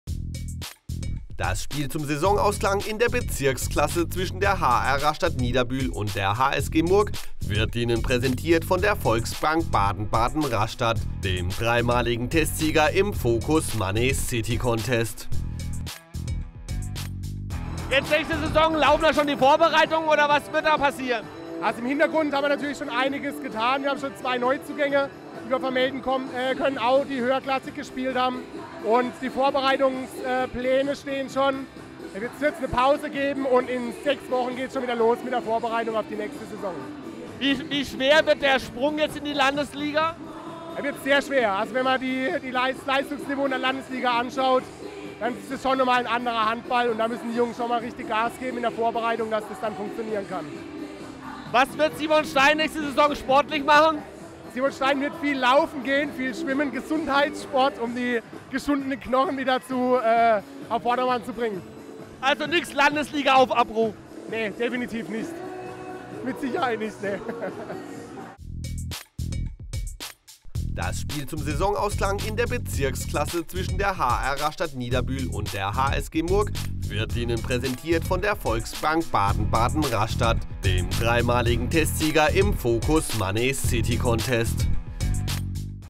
Ligaweb.tv - Simon Stein im Interview
Simon Stein vom Landesliga-Aufsteiger HR Rastatt/Niederbühl über seine sportliche Zukunft in der nächsten Saison